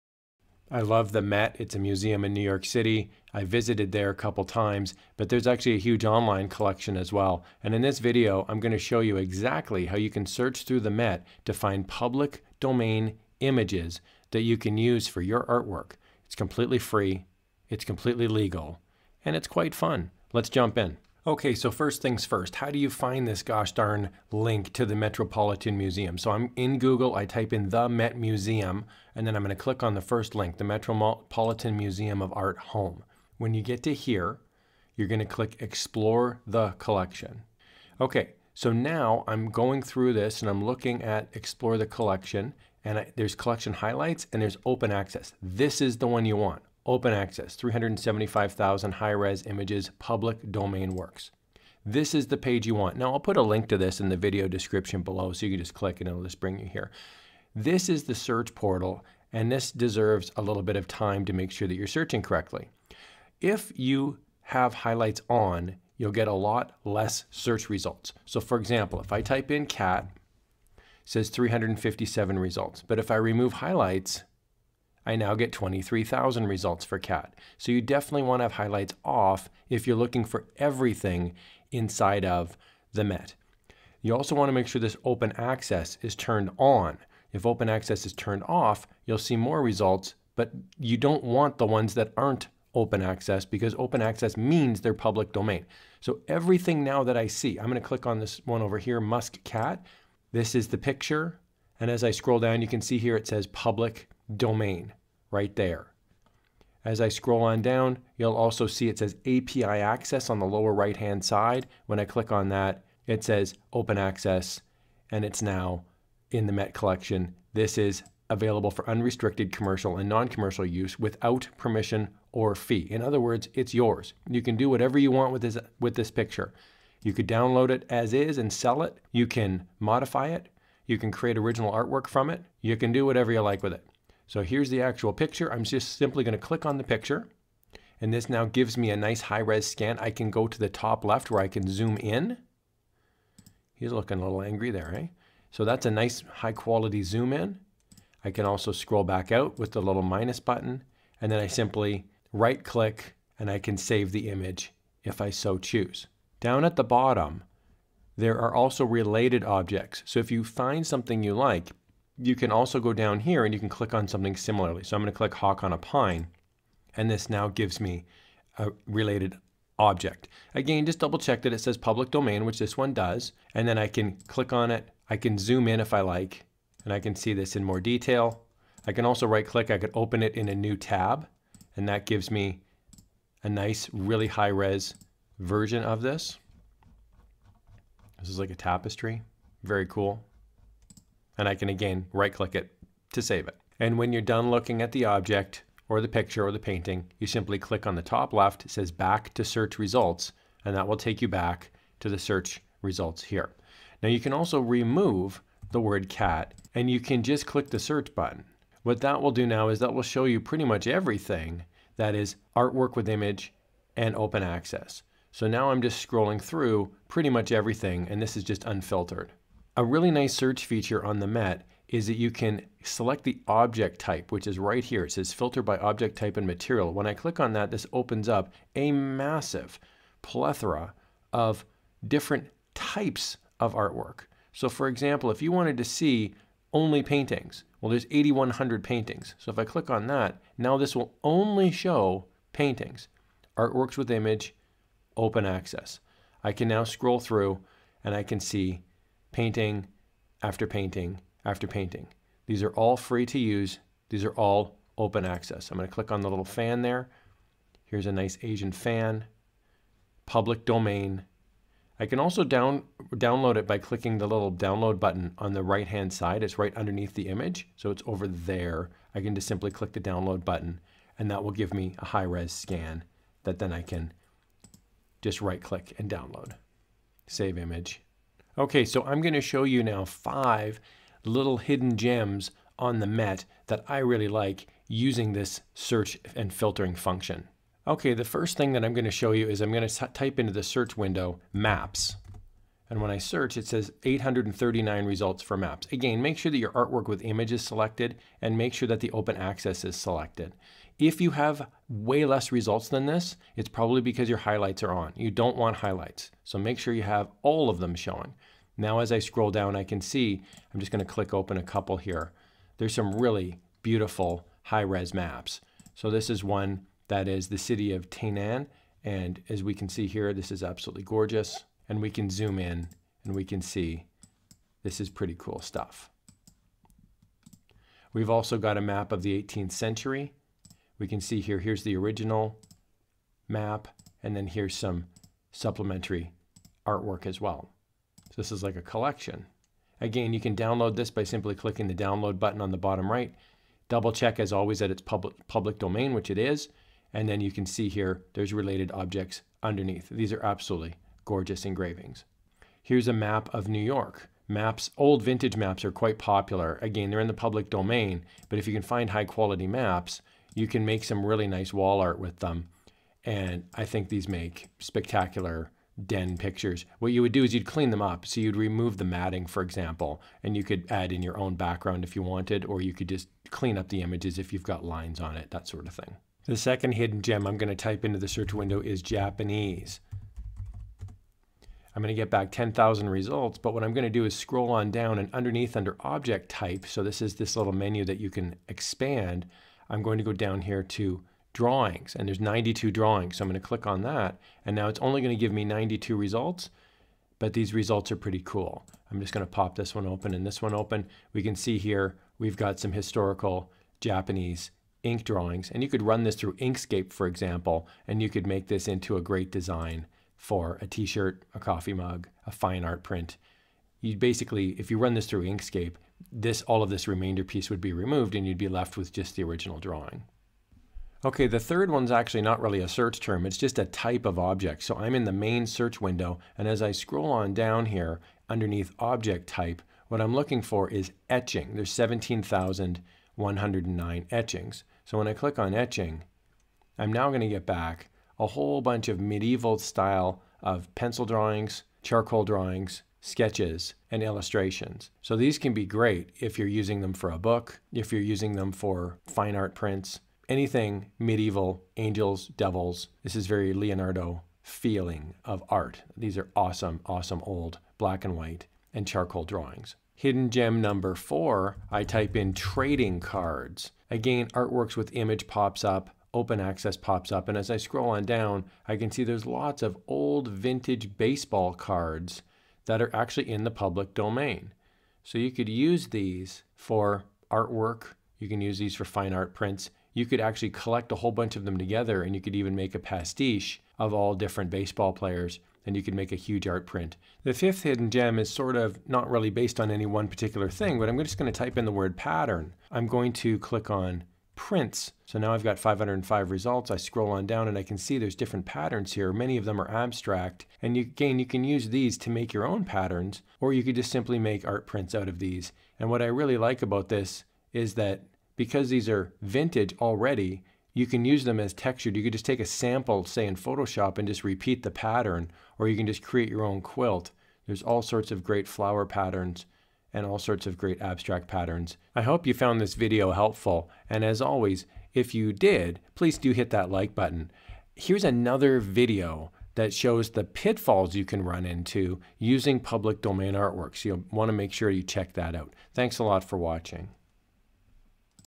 THE MET OPEN ACCESS Tutorial (Public Domain Images For Commercial Use)
The Metropolitan Museum of Art ("The Met") has over 375,000 images that are considered 'open access', which means that you can use them for any commercial or non-commercial purpose without attribution or fee. Basically, they can be used for any reason! The hard part is navigating the huge website and trying to find specific images. In this video, I do a walkthrough of the Met Open Access search portal and show off 5 hidden gems that can help spark your searching on the Met! These designs can be great raw fodder for your print-on-demand artwork (Redbubble, Teepublic, Etsy, etc.). It can also be a useful tool if you are creating digital artwork for sale on Etsy or other digital platforms.

Timestamps:
00:00 What is Open Access?
00:21 How To Find It
00:51 How To Search
04:47 Hidden Gem #1
06:16 Hidden Gem #2
07:00 Hidden Gem #3
07:40 Hidden Gem #4
08:07 Hidden Gem #5